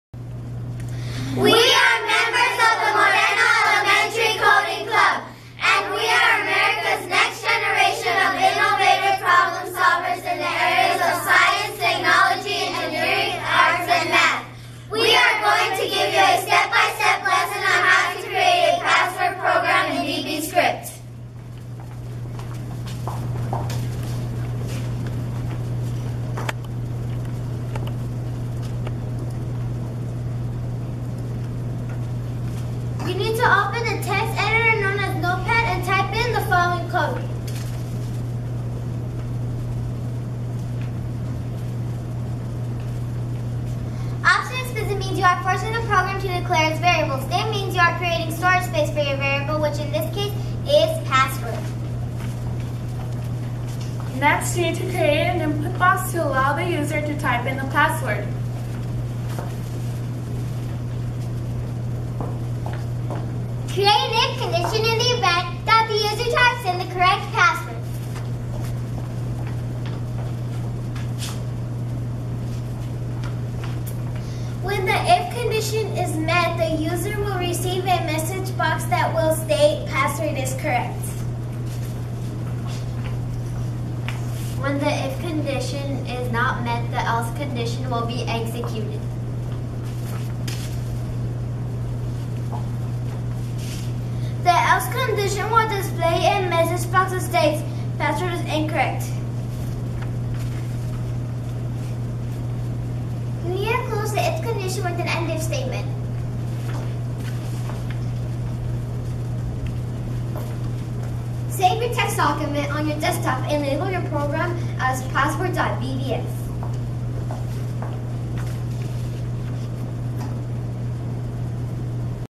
Moreno Elementary Coding Club
The Coding Club at Moreno Elementary goes through and shows how to create a password program.